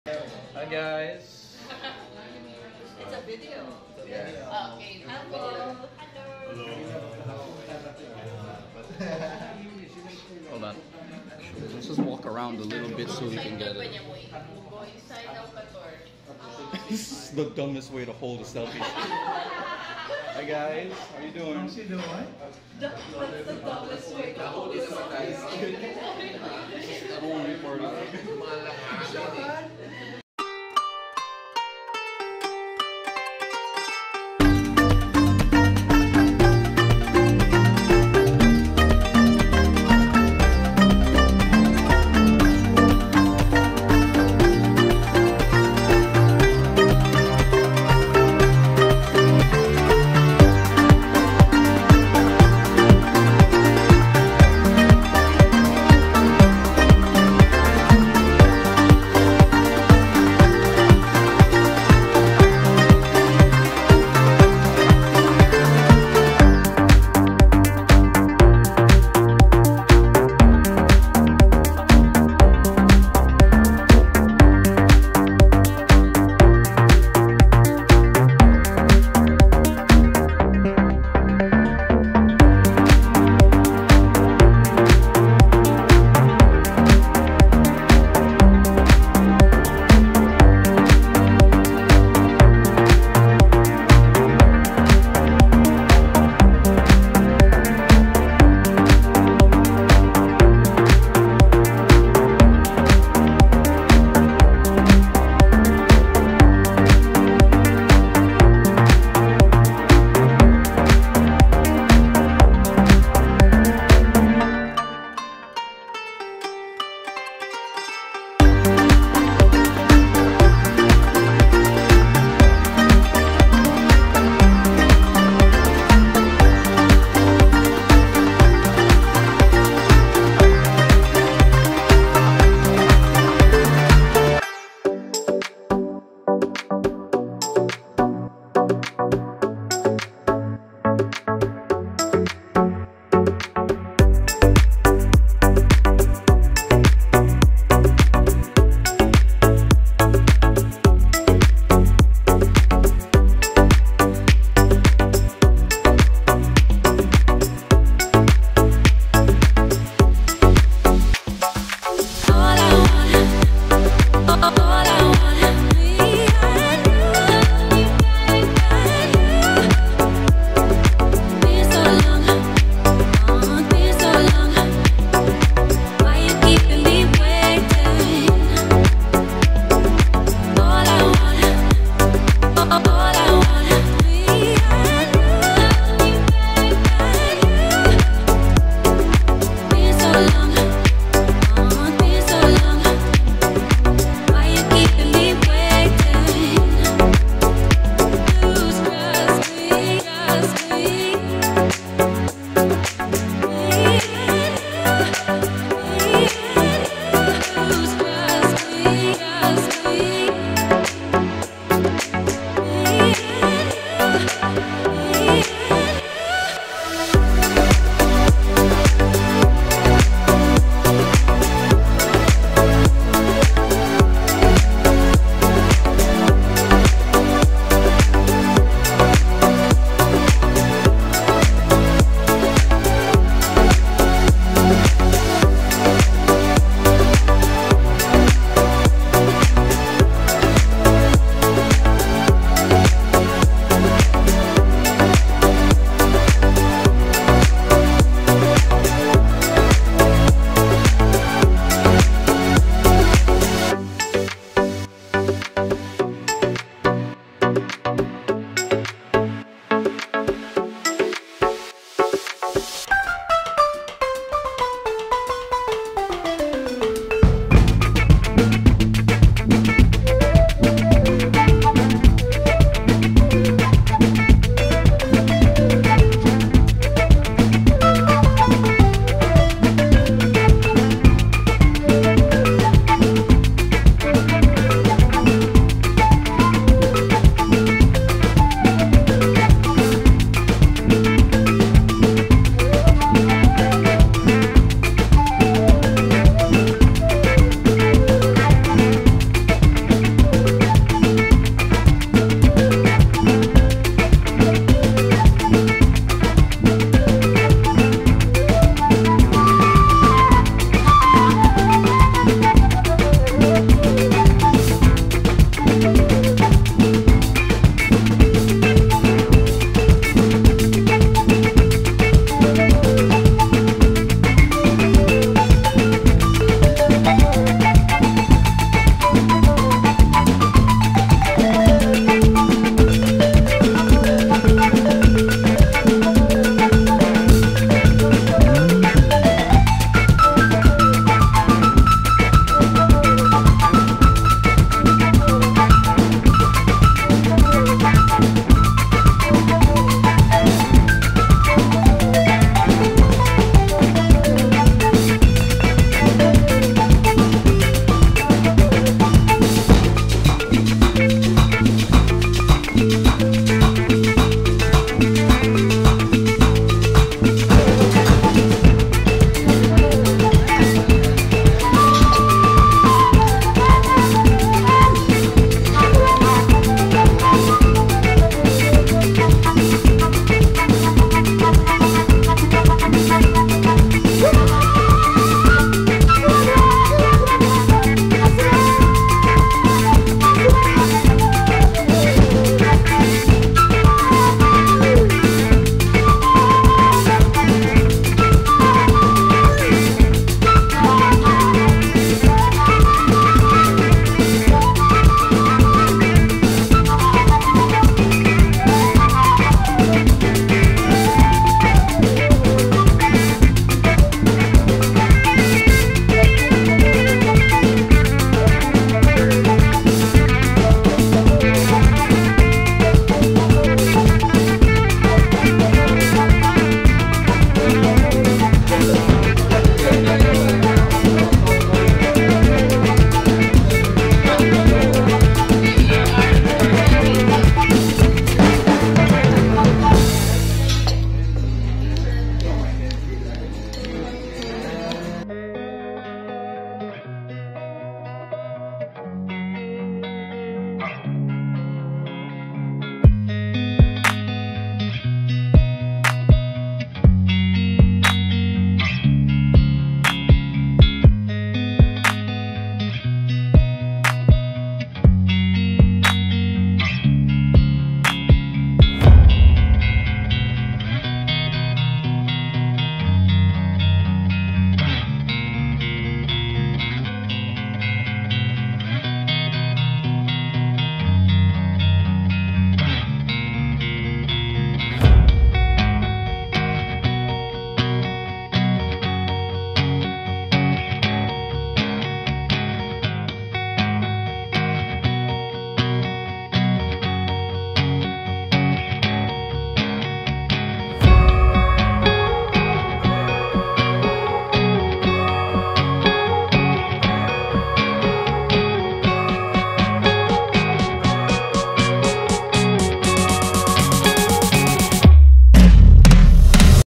Authentic Spanish Restaurant
Ipar's Authentic Spanish Restaurant is one of the best spanish restaurant here in Cebu.
If you’re driving around the city, it’s an essential stop to taste their great classics of Spanish cuisine at an extremely reasonable price. See you!